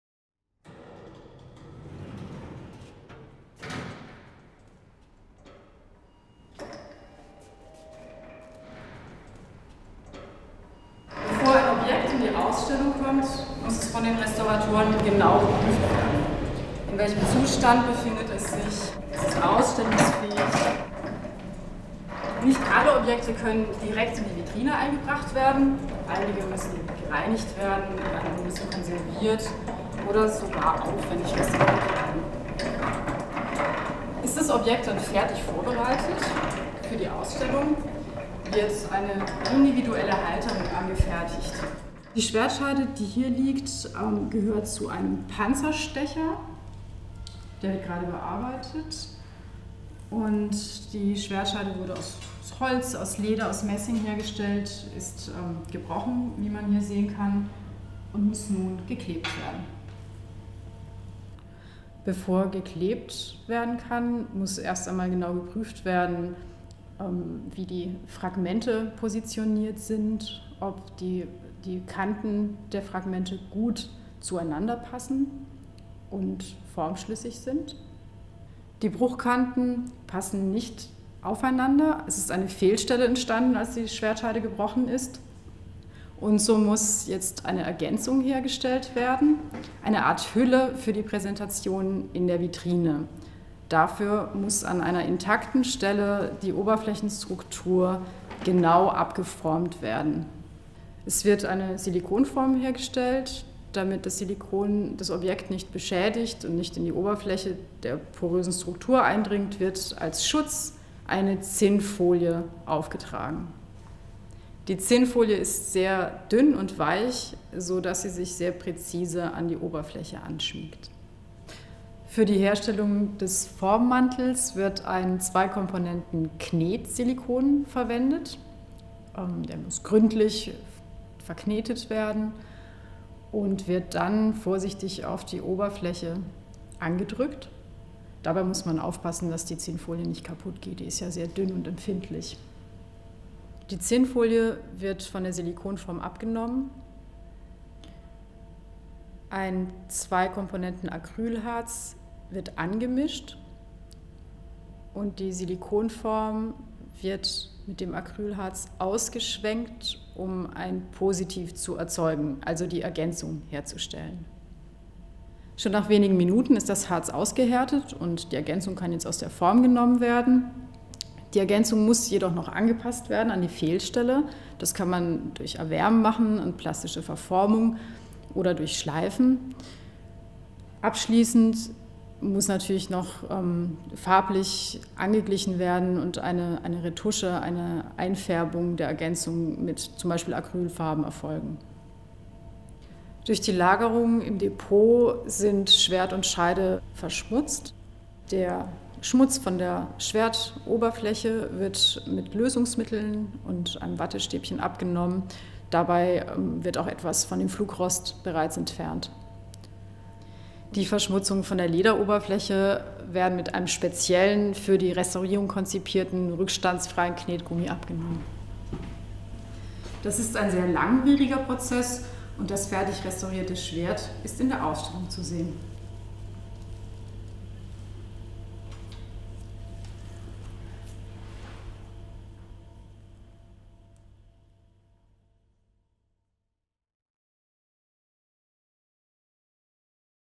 Faszination Schwert | Einblicke in die Restaurierung | Landesmuseum Württemberg, Stuttgart
Für die Große Sonderausstellung „Faszination Schwert“ haben wir mehr als 340 Objekte ausgewählt. Alle diese Objekte wurden genau geprüft und bei Bedarf überarbeitet oder repariert, damit sie "fit" für die Präsentation sind. Wie eine solche "Restaurierung" aussehen kann, zeigt dieses Video.

Die Große Sonderausstellung „Faszination Schwert“ zeigt vom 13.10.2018 bis zum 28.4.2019 erstmalig die ganze Erfolgsgeschichte des Schwertes: Von seinem ersten Einsatz in Mitteleuropa im 2. Jahrtausend vor Christus bis hin zu weltweit bekannten Schwertern in Serien und Computerspielen.